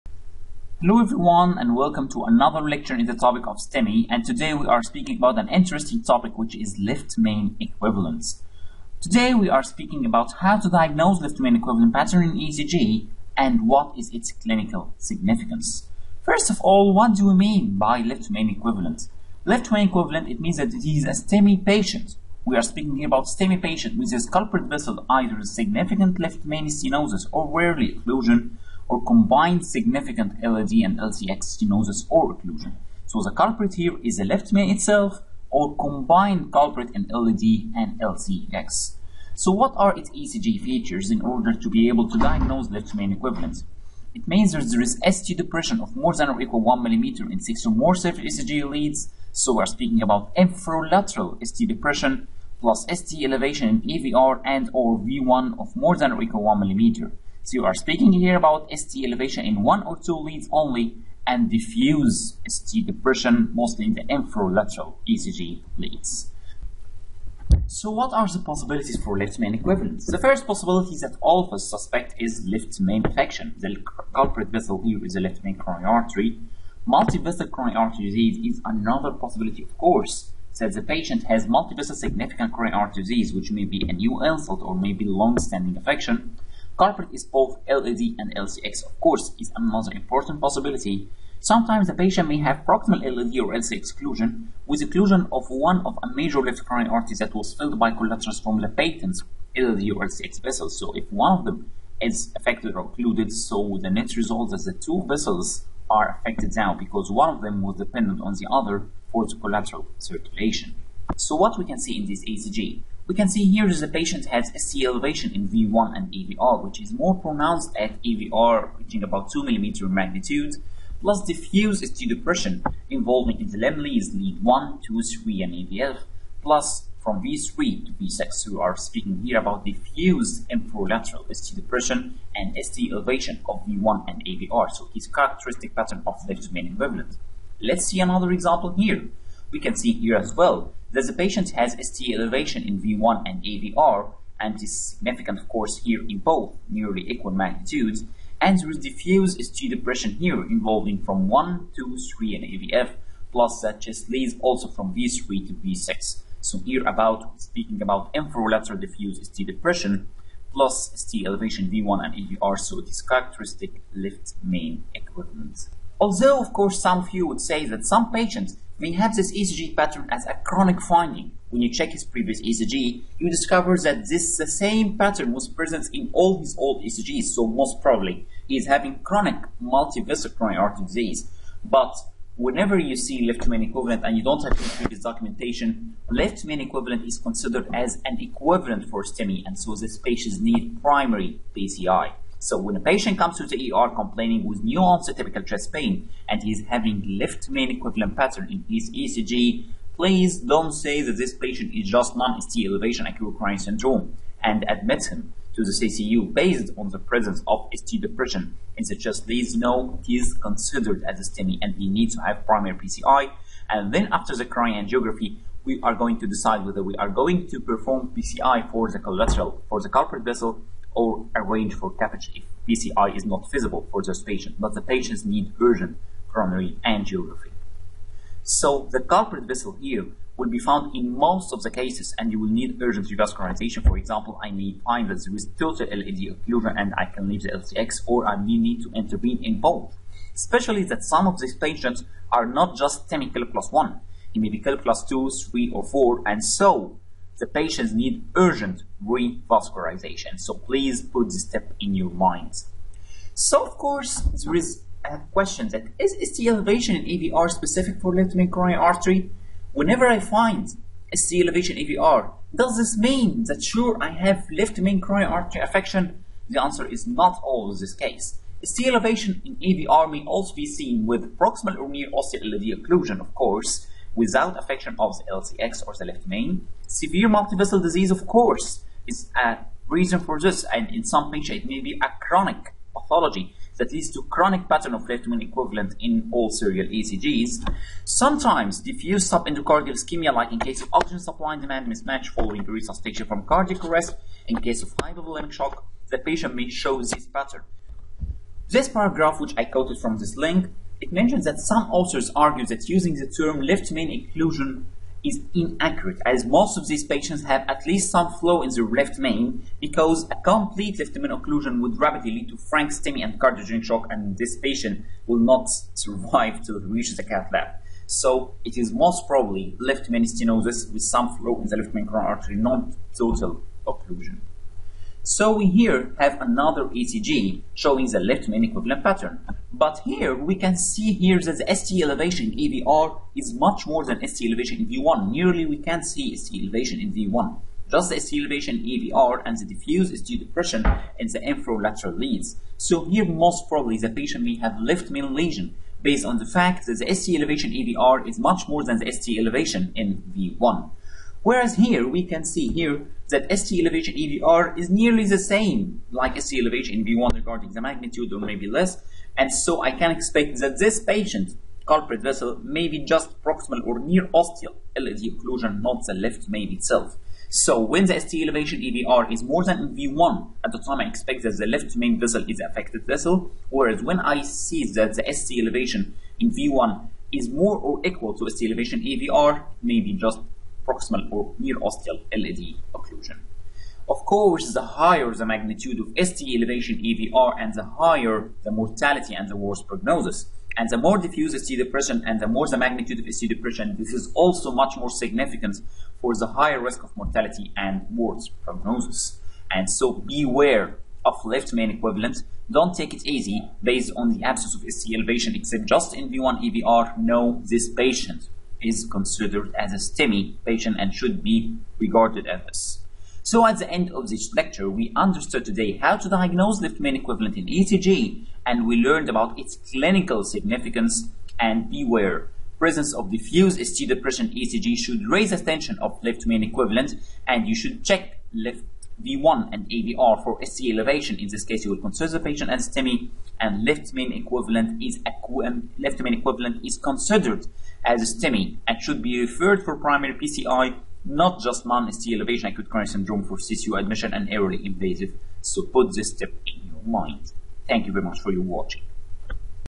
ECG course: Left Main Equivalent, Dr. Sherif Altoukhy
This lecture explains the ECG features of left main equivalent and its clinical significance in order to facilitate its detection in the clinical practice.